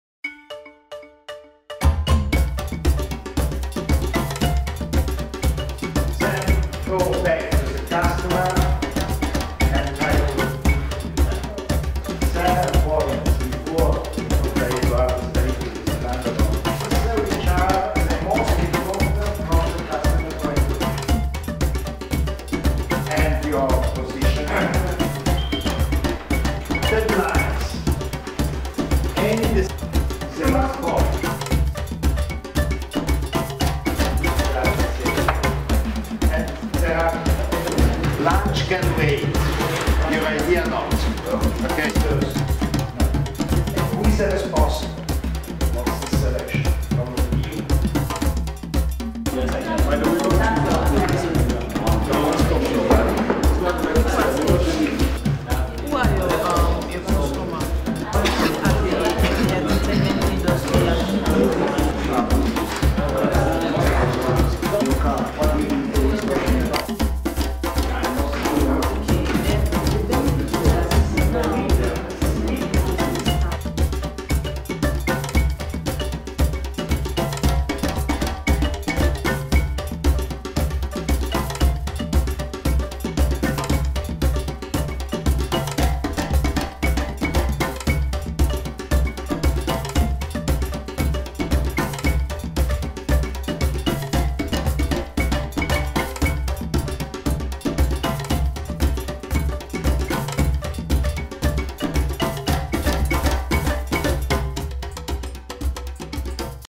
E4Impact Long Weekend in Makeni
How is a long weekend at the E4Impact MBA? Entrepreurial courses, knowledge sharing, international professors and much more. E4Impact: we do not teach entrepreneurship, we train impact entrepreneurs!
Starring: class of University of Makeni (Sierra Leone", 1st edition MBA)